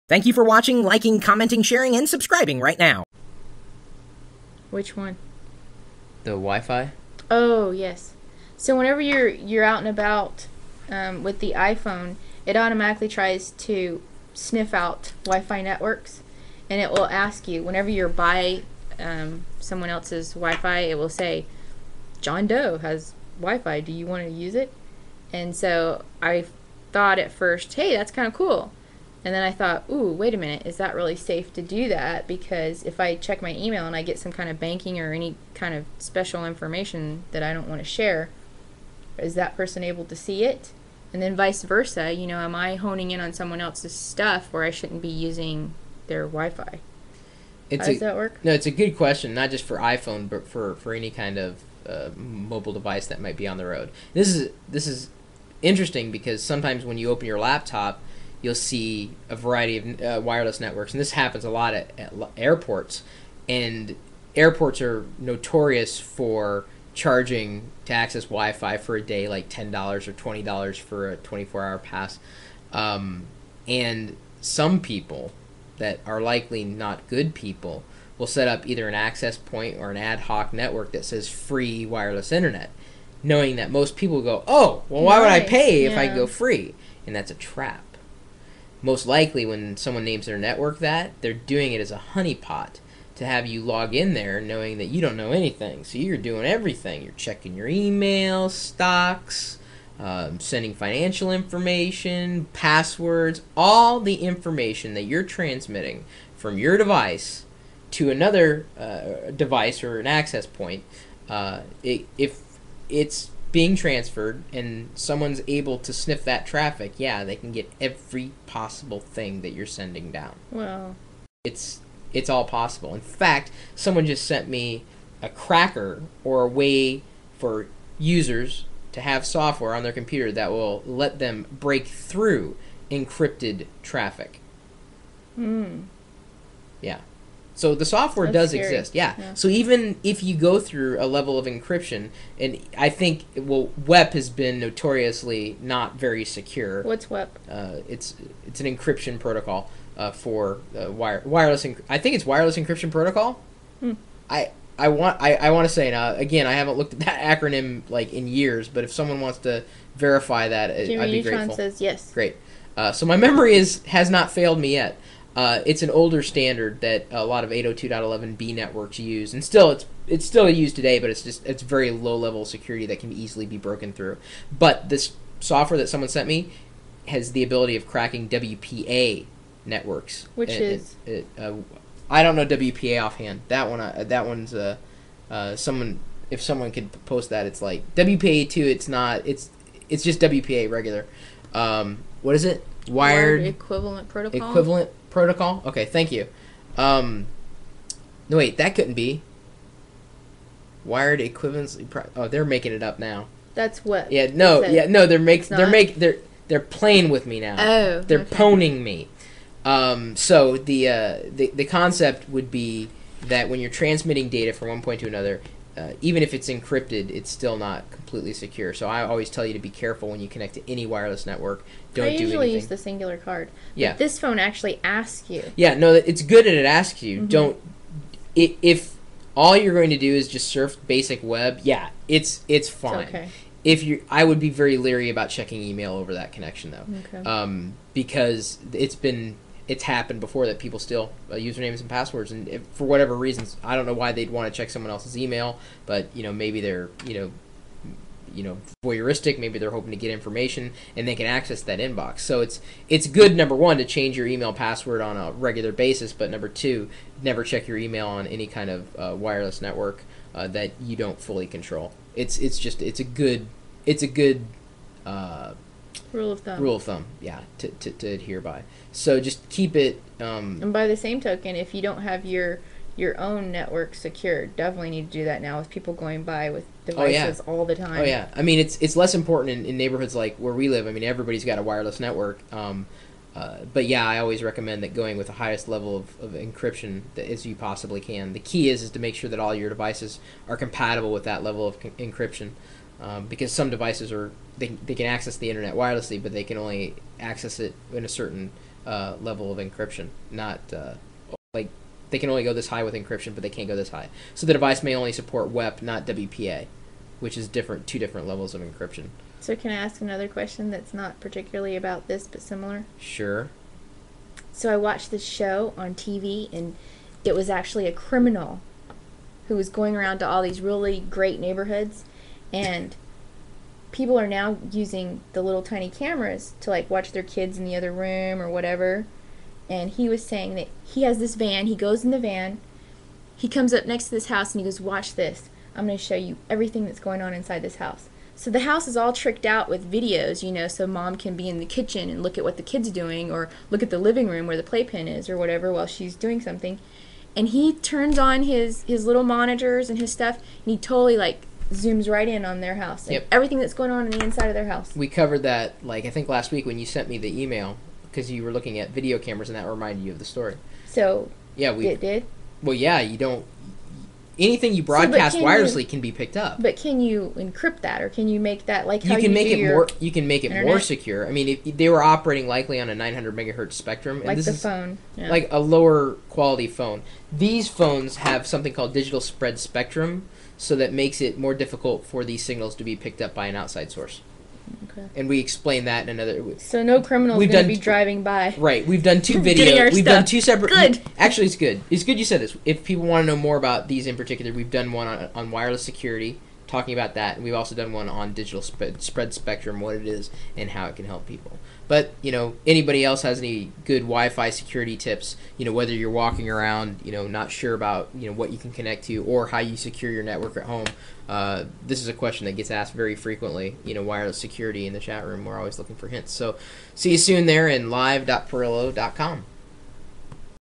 WiFi Packet Sniffing on iPhone
/ - WiFi is an amazing utility for geeks on the go, but is it safe to use an open WiFi access point?